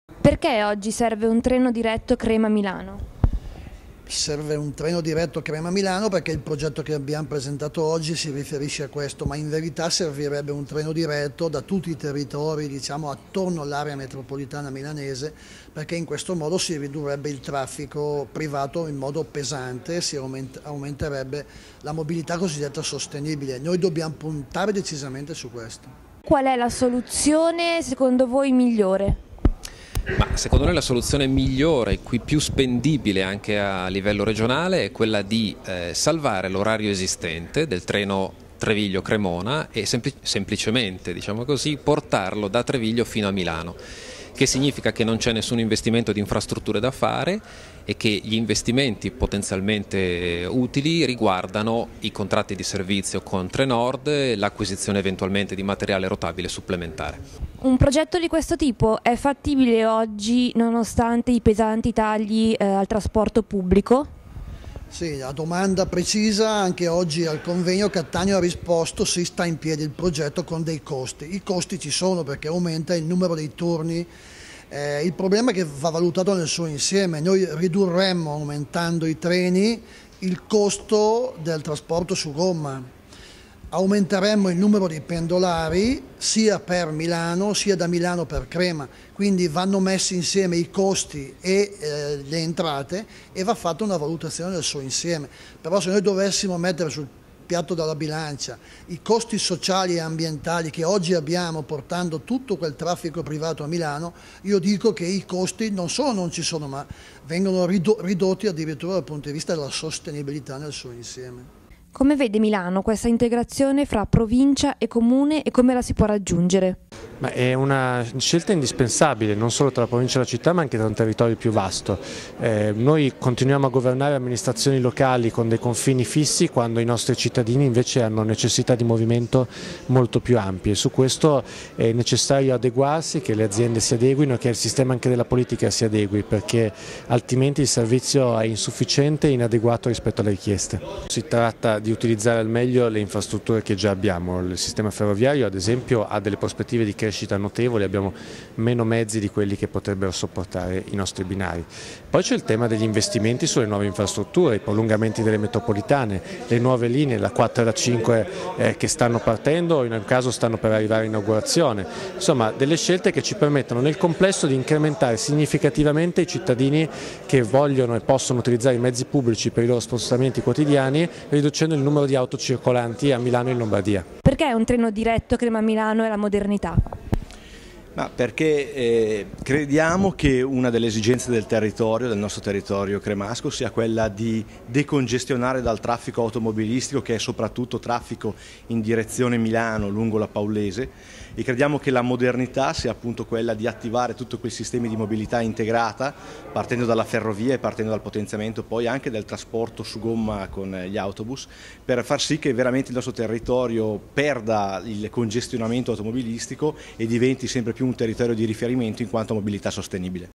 Treno diretto Crema-Milano? Si può
Presentato il libro promosso da Agostino Alloni (PD): "Treno diretto Crema-Milano. In viaggio verso la modernità"